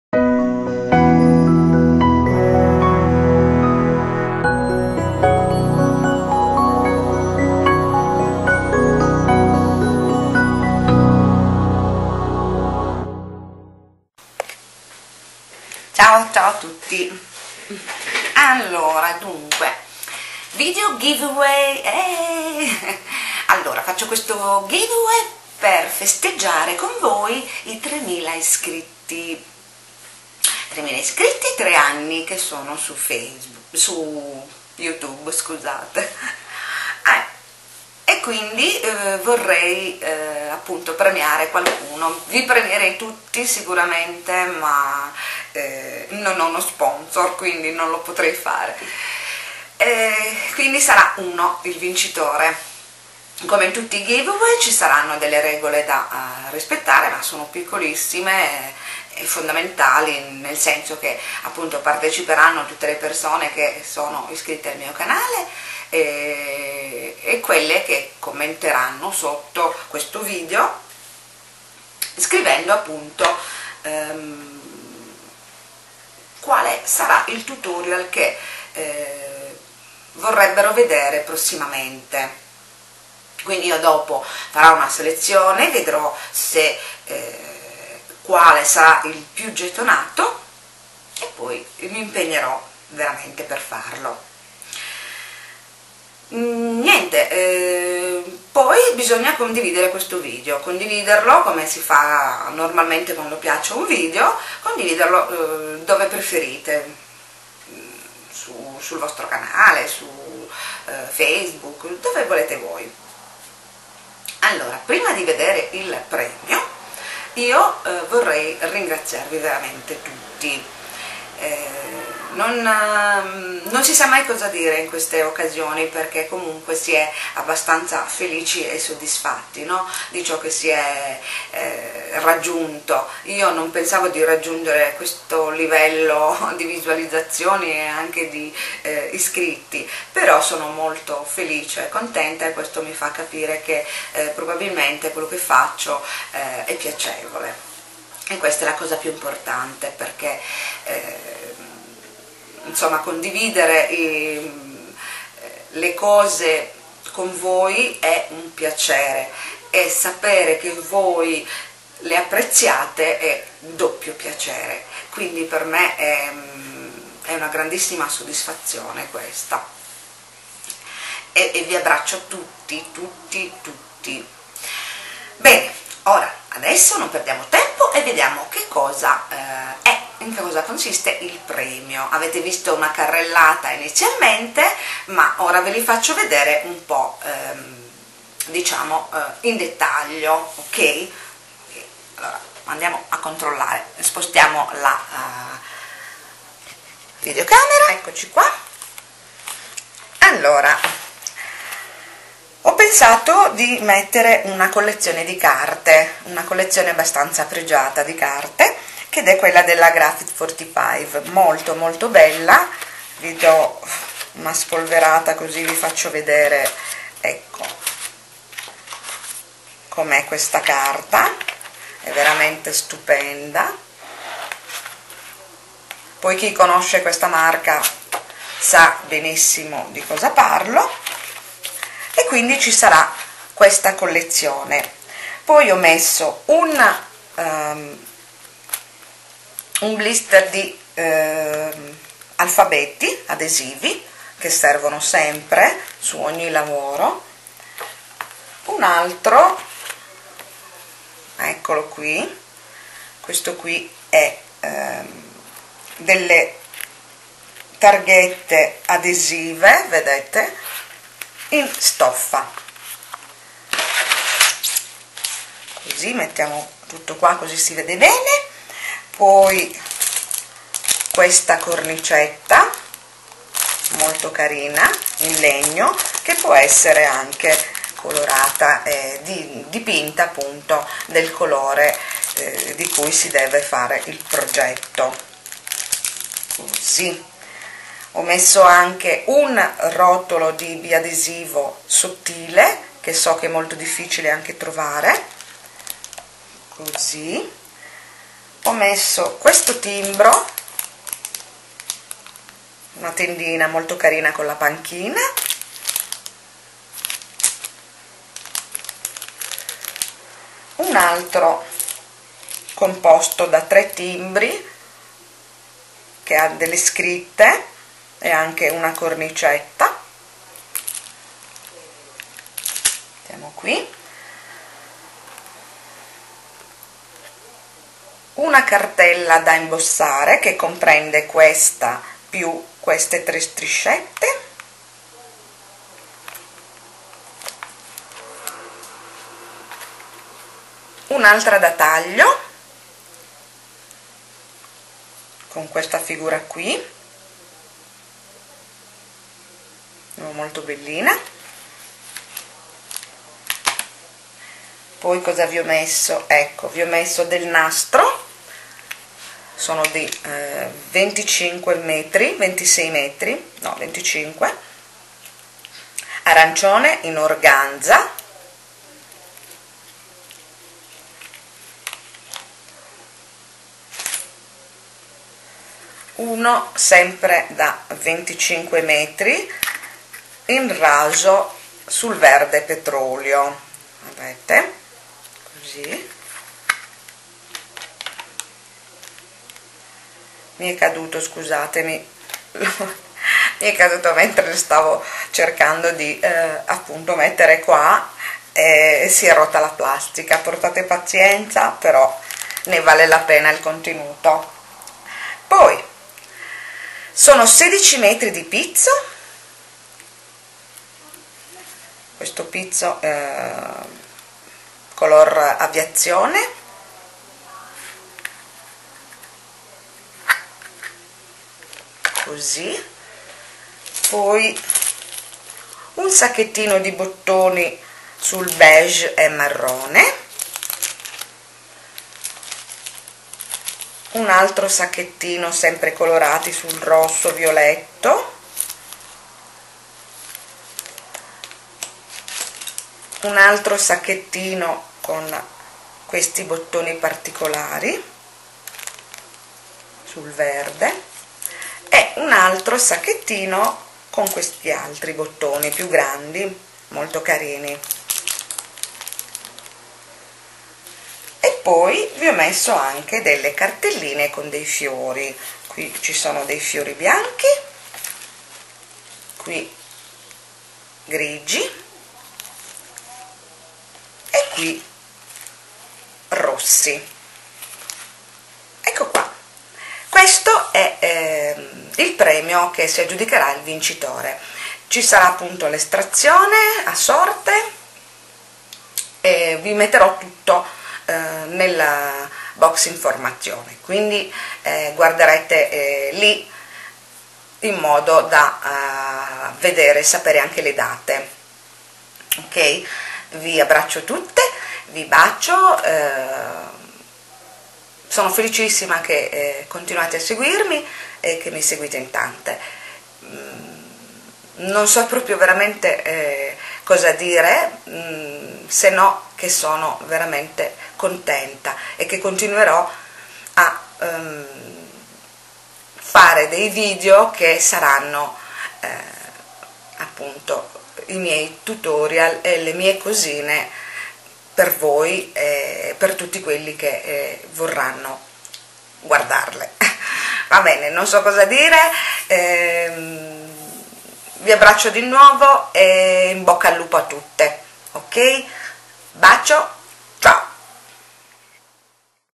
Giveaway 3000 iscritti!!!! CHIUSO
Regole per partecipare
1) essere un mio iscritto al canale
2) commentare questo video dicendo quale tutorial vorreste vedere
3) condividere questo video
Le adesioni finiscono il 22 e l'estrazione averrà il 23 tramite Random. org.
Vi abbraccio e buona fortuna .
Ps. un solo commento grazie